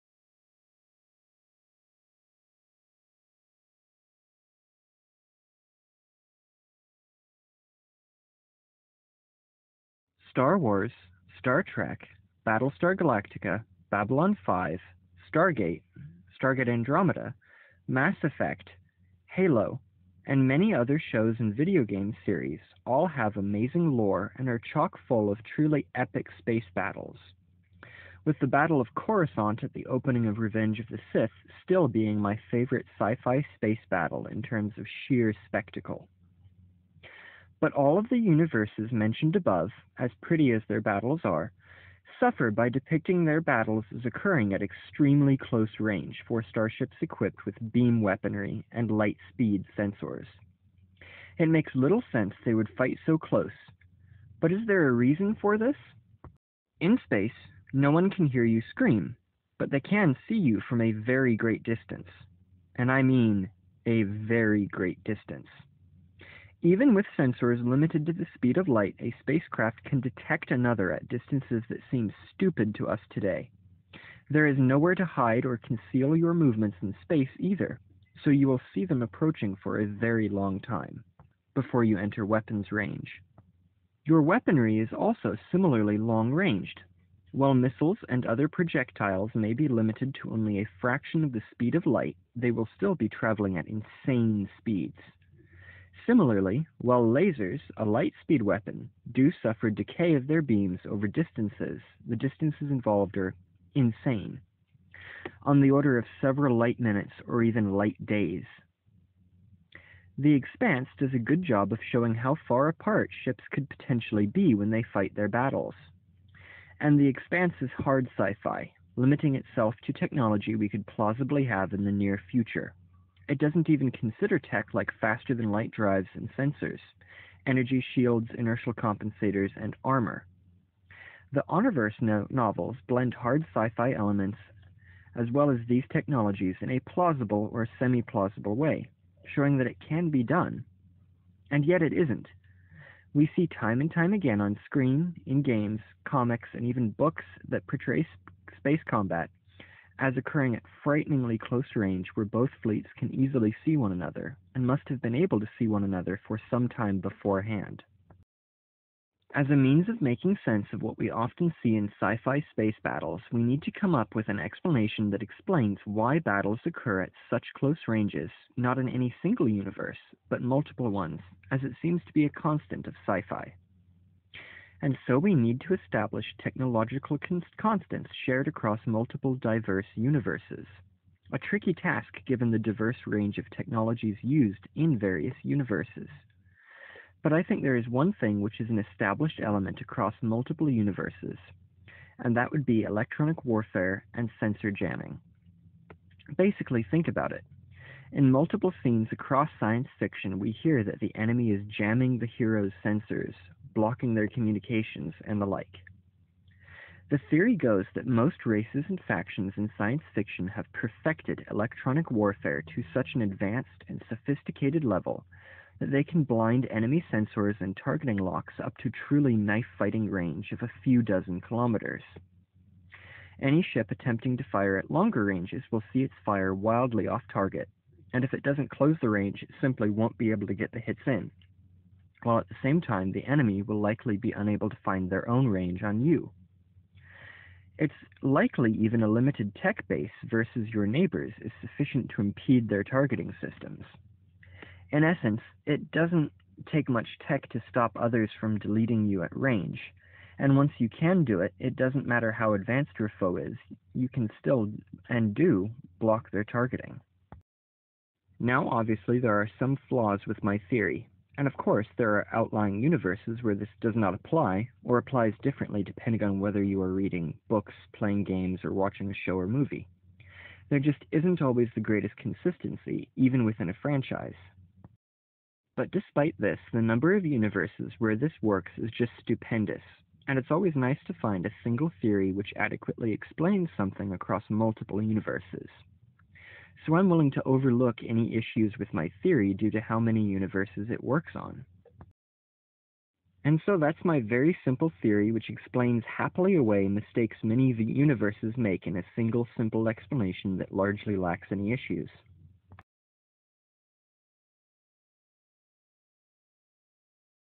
Why are sci-fi battles fought at such close range? (Theory)
In this video we look at possible reasons why many sci-fi battles happen so close together.